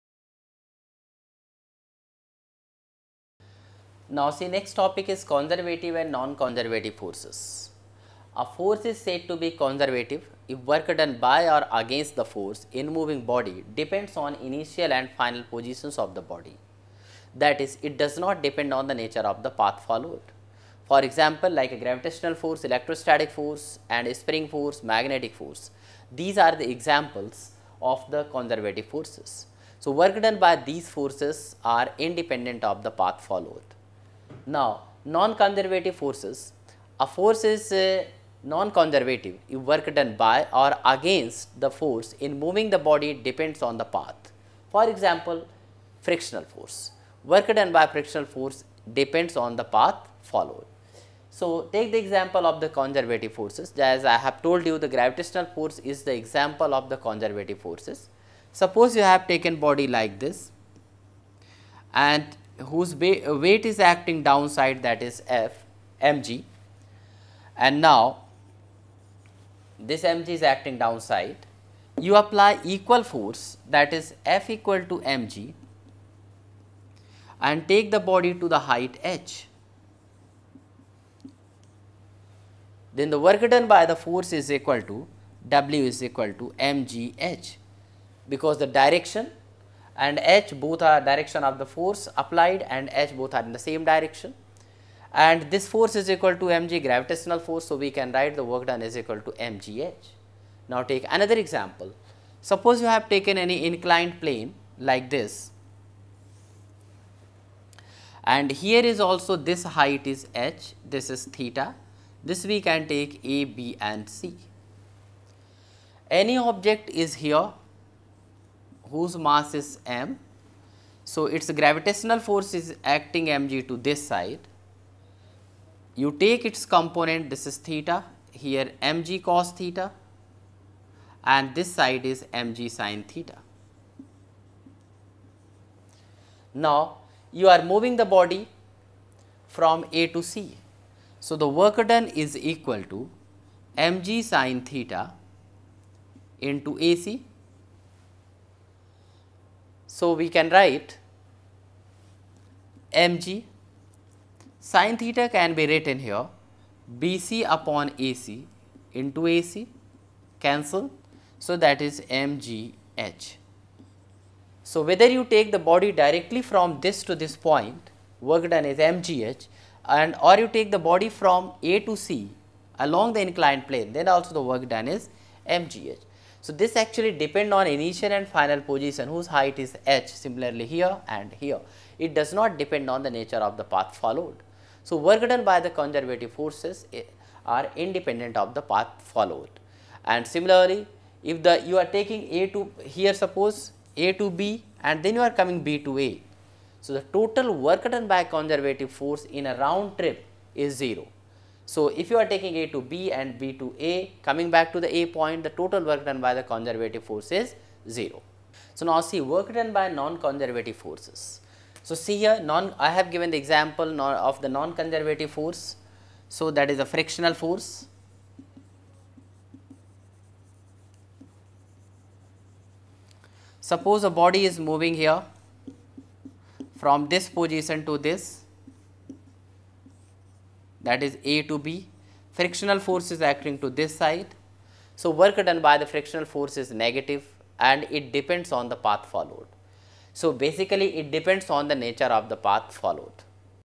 CBSE Class 11 Physics Work,Energy & Power Conservative and Non-Conservative Forces |Extraminds
Chapter: Work,Energy & Power
Topic: Conservative and Non-Conservative Forces
Extraminds